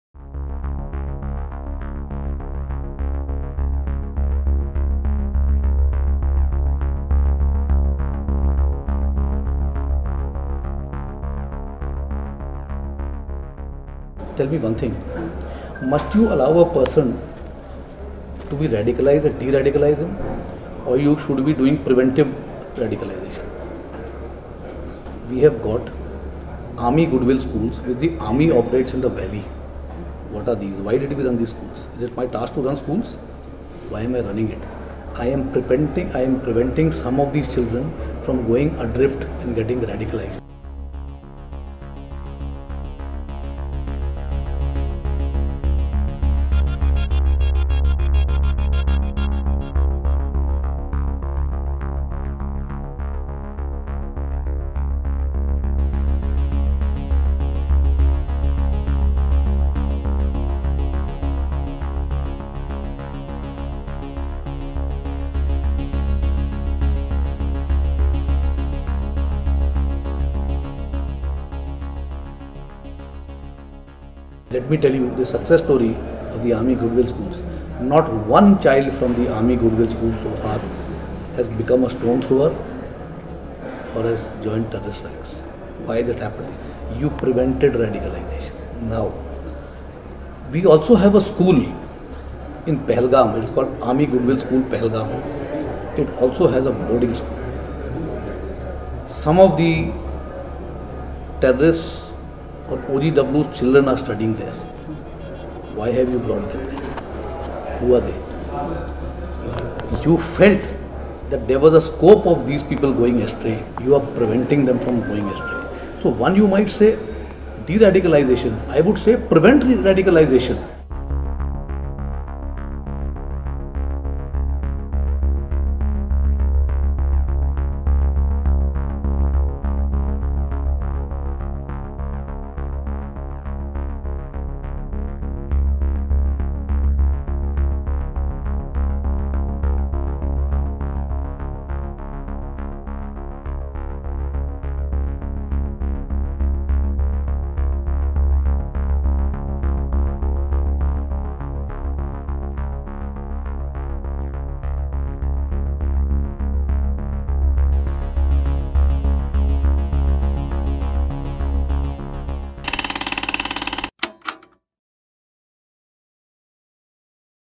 Army's Goodwill Schools in Kashmir Are ‘Preventive Radicalisation Camps’, Says CDS Bipin Rawat
The Goodwill schools that the Indian Army runs in Jammu and Kashmir are essentially ‘preventive radicalisation camps’, India’s newly recruited Chief of Defence Staff, General Bipin Rawat, has told our sister channel, News18 in an exclusive interview.
Explaining his controversial remark at the Raisina Dialogue that young children being radicalised in Kashmir need to be identified and placed in de-radicalisation camps, Rawat once again emphasised the need for ‘preventive radicalisation’.